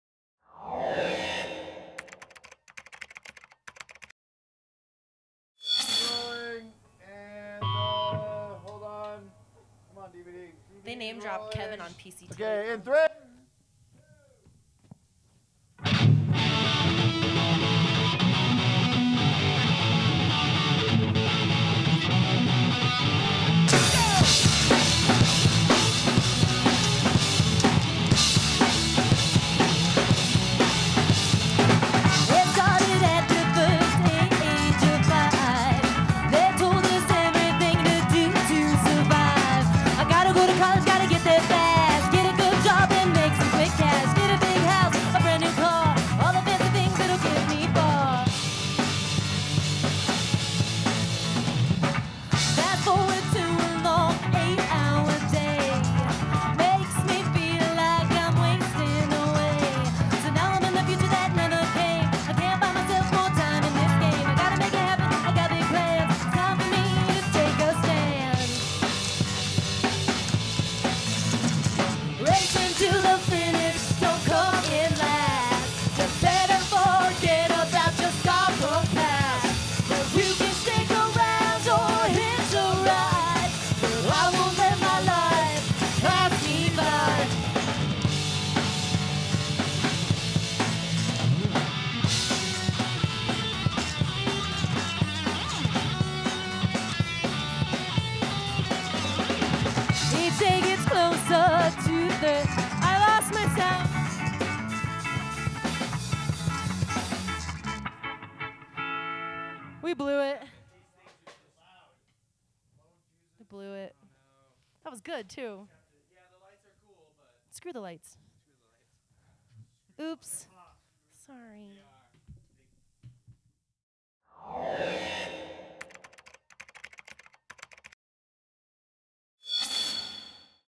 Bombshell Academy Outtake
Bombshell Academy rocked a little to hard during the taping of one of their songs, and blew a fuse! Here's the outtake.